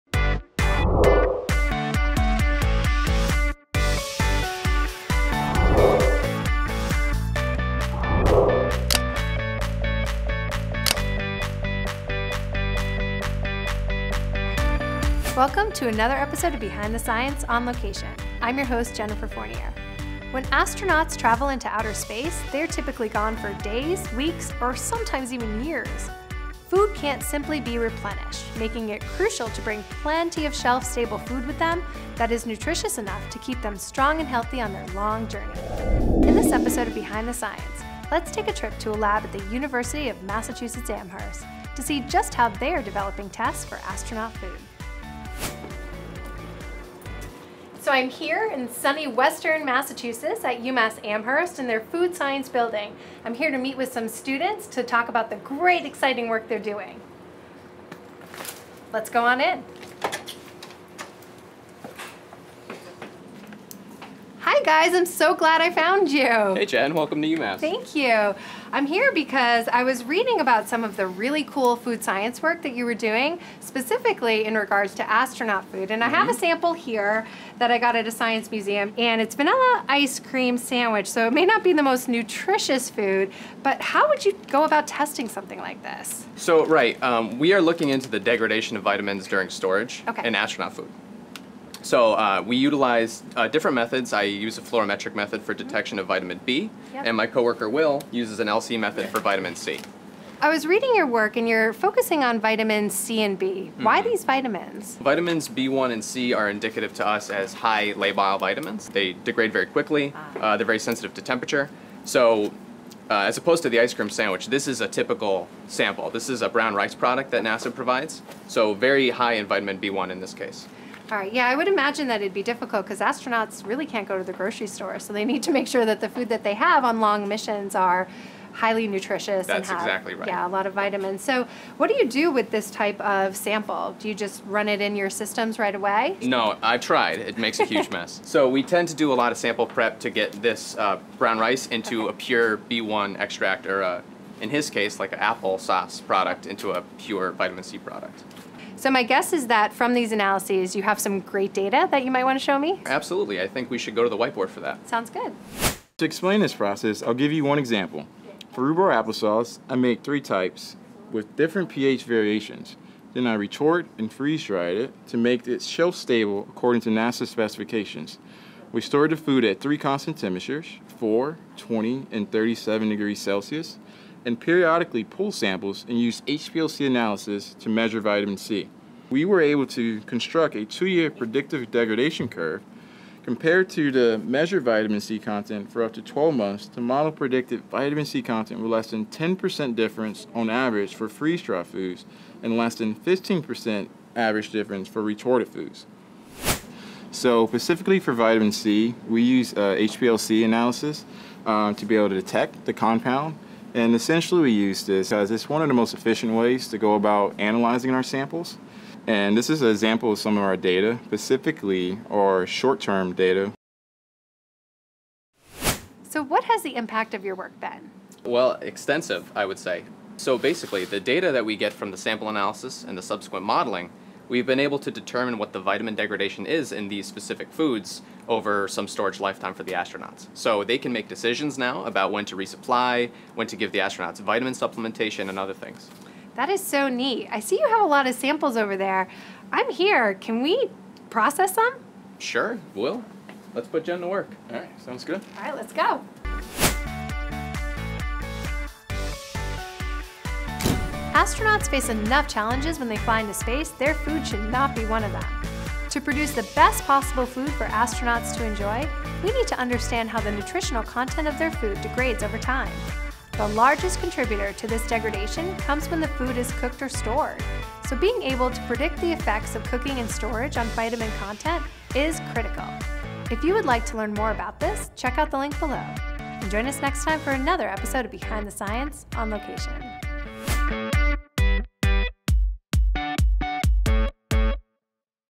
How do scientists at UMass-Amherst make sure astronauts' food is nutritious? | Behind the Science
Since there aren't any grocery stores in outer space (yet), astronauts have to bring shelf-stable and nutritious food with them. In BehindtheScience, Jen visits the University of Massachusetts at Amherst to meet Tim Goulette and William Dixon, food science Ph.D. students. They're researching the degradation of vitamins B and C in astronaut's food such as brown rice and rhubarb apple sauce during storage. For vitamin C, the researchers use HPLC data to model vitamin performance over a variety of storage lifetimes, so agencies can make decisions about when to resupply food and when to use vitamin supplements. For more information about how vitamins are analyzed, go